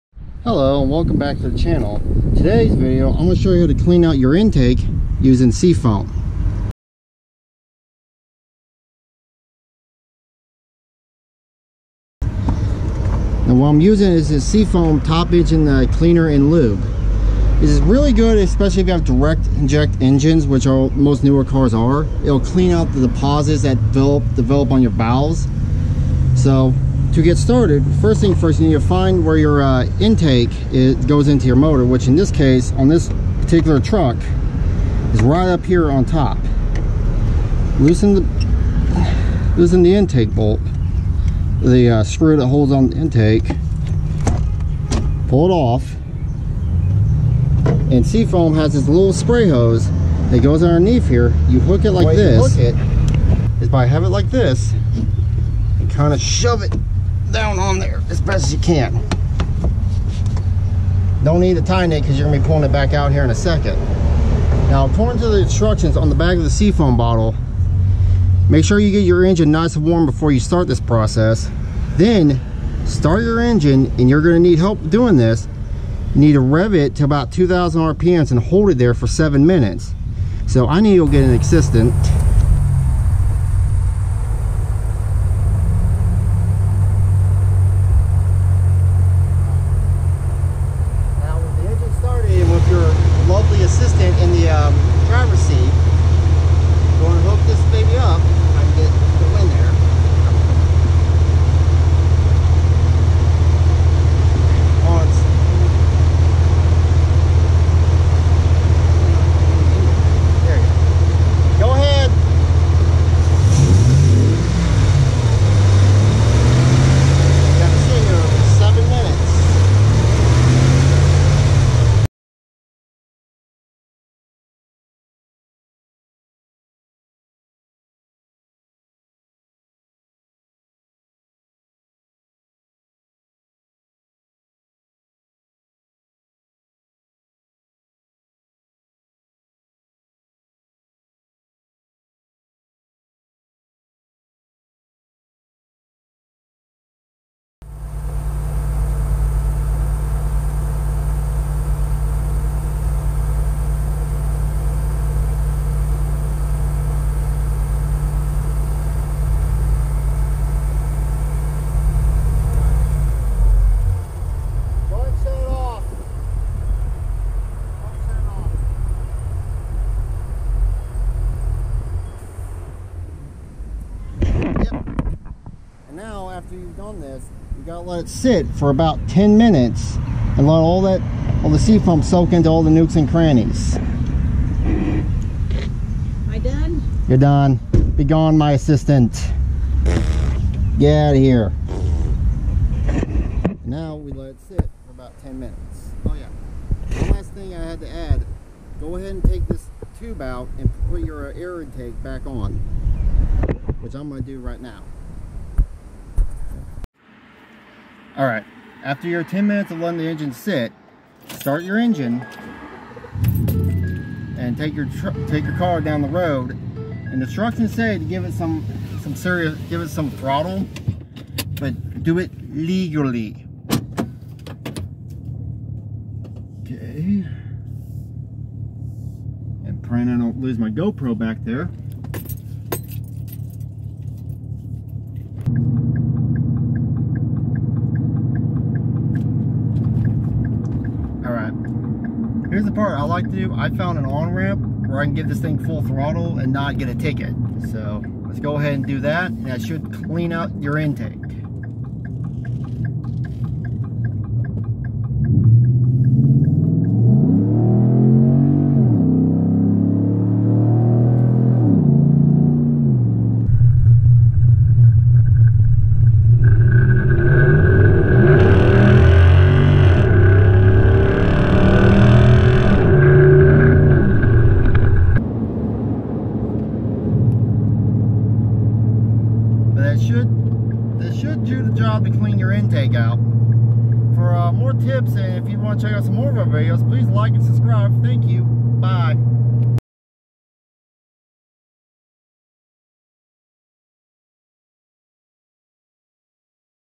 How to clean your Intake using Seaform
In this video I show how to use Seaform to clean your intake and valves.